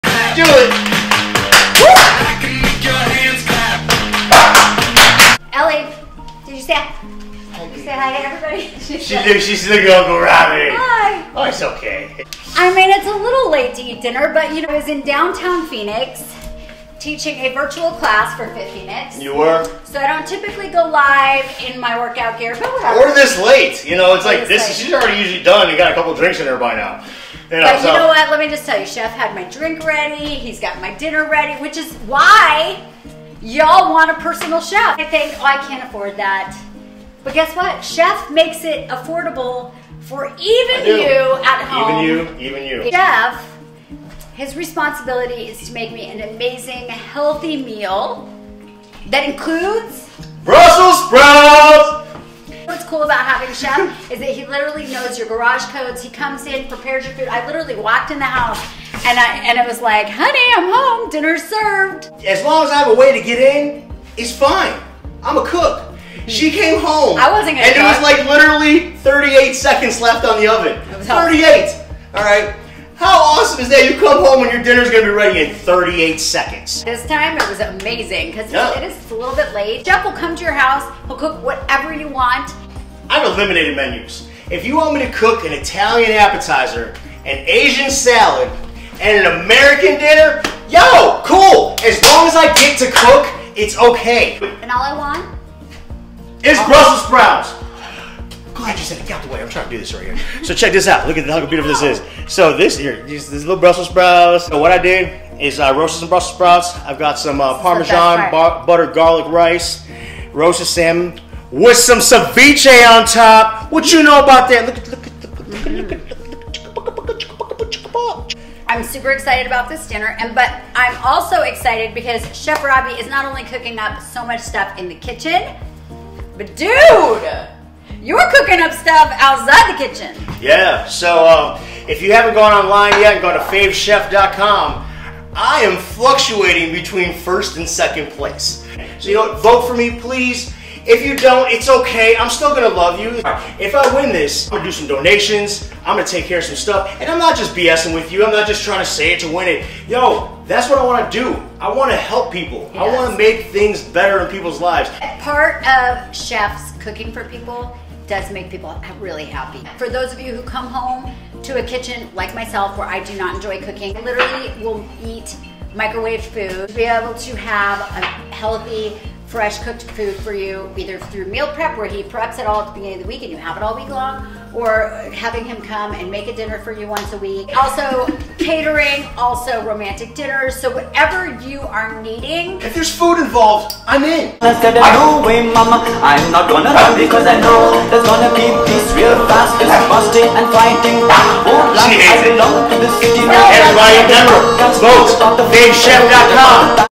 Health Held in Hand .. your own personal chef
Chef Robbie @healthheldinhand is the personal chef you have always wanted. affordable for everyone. he charges hourly plus gratuity... he does your shopping, preps, cooks, cleans and leaves. meal prepping, catering, dinner parties, romantic dinners and more... u name it, he will do it!!! Save 20% on your first meal service when u mention lifestylejules go or call to schedule your dinner to vote for Chef go